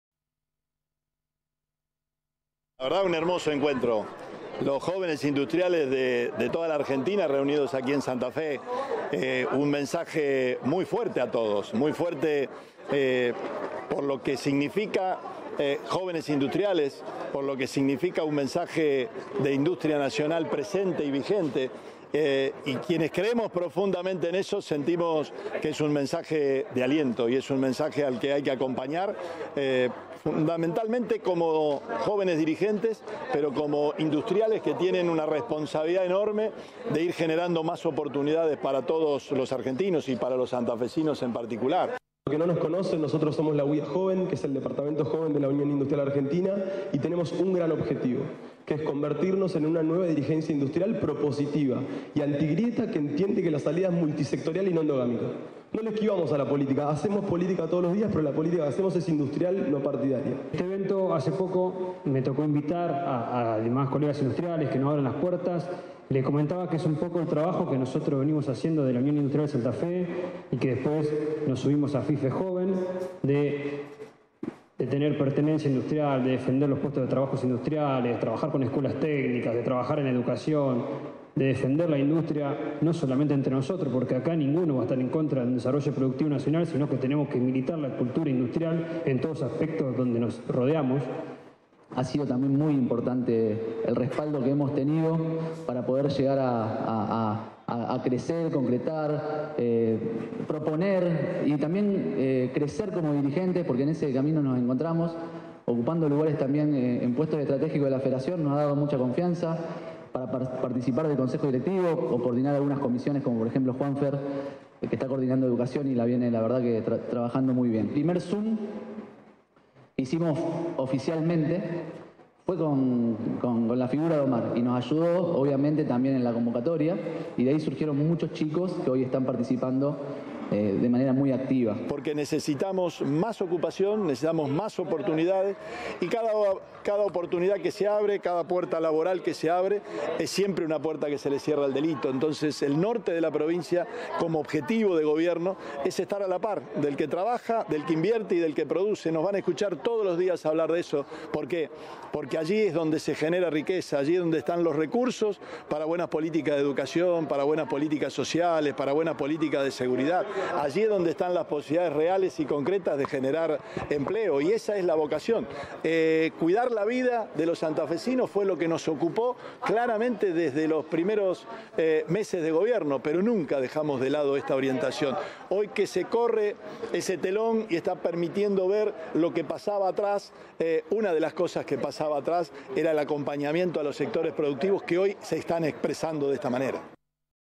Se realizó el primer Tour Industrial por Santa Fe y Entre Ríos
Los jóvenes de la UISF Joven recibieron a más de 70 representantes de diferentes puntos del país nucleados en UIA Joven.